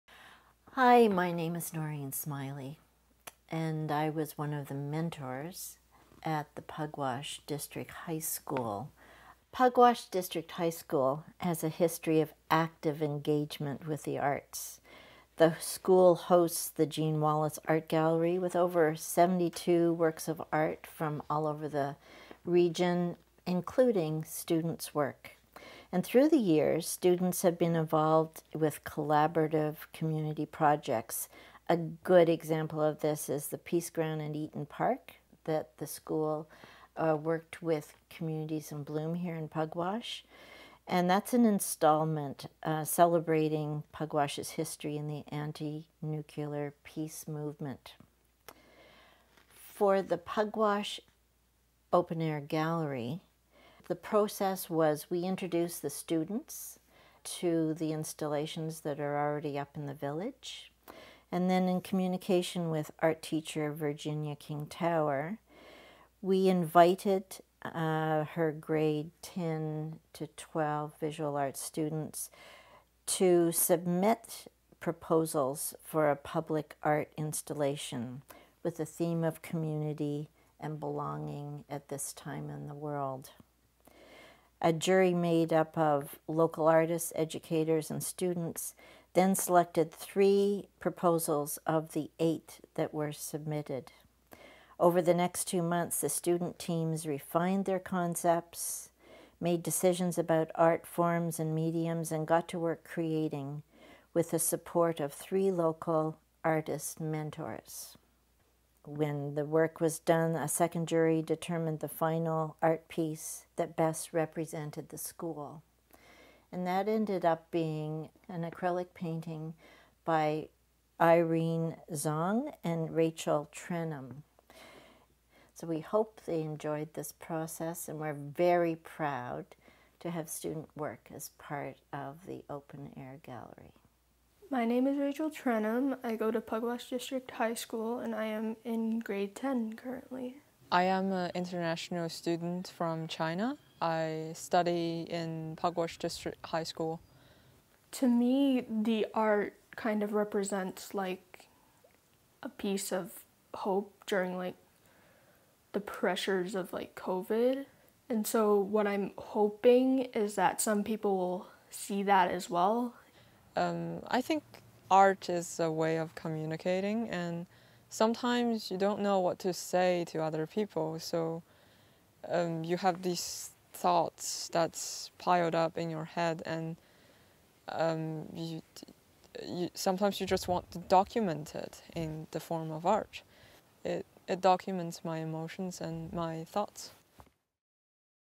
Pugwash District High School
Visual arts students in grades 10 to 12, submit art installation proposals. Three teams develop their concepts with artist mentors and make their final art pieces. A jury of educators students and local artists select the art piece that will represent the Pugwash District High School in the Pugwash Open Air Gallery.